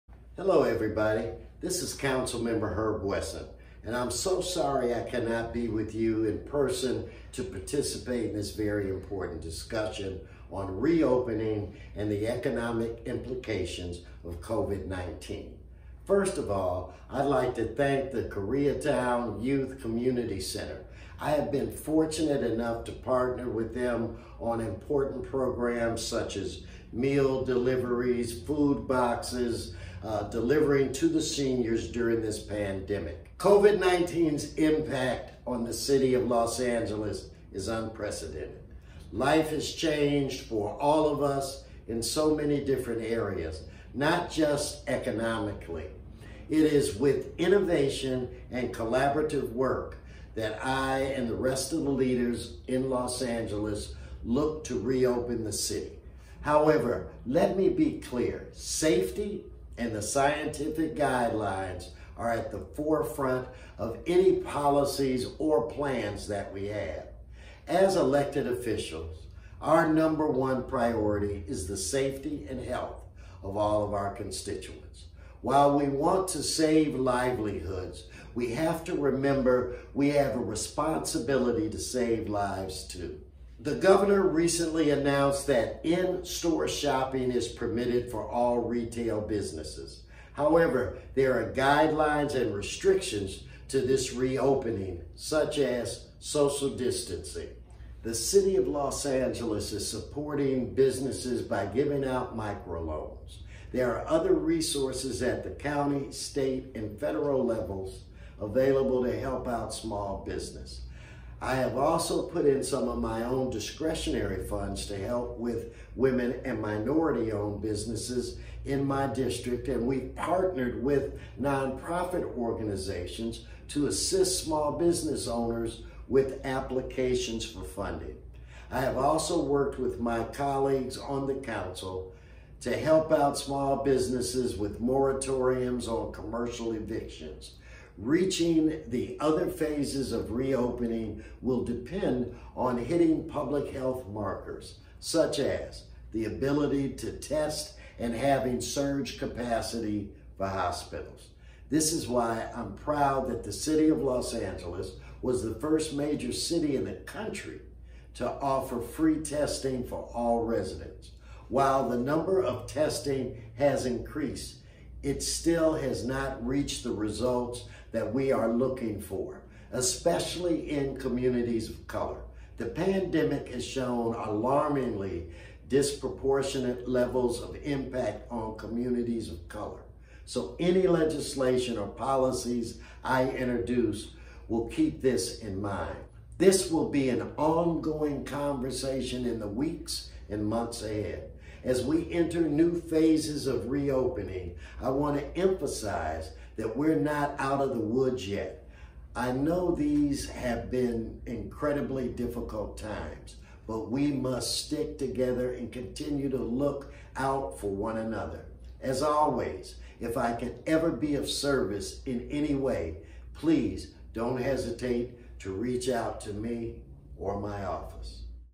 Councilmember Herb Wesson's Message at KYCC's '(Re)opening For Business' Webinar Series
KYCC's Small Business Program is launching a three-part webinar series IN KOREAN on how small businesses can navigate the changing landscape of a post-COVID environment. The first webinar of the series took place on May 27 at 2:00 p.m. with a special message from Los Angeles City Councilmember Herb Wesson of Council District 10.

What will the reopening look like? How can I support my employees—what are the labor laws? And how can I better market my business in the near future?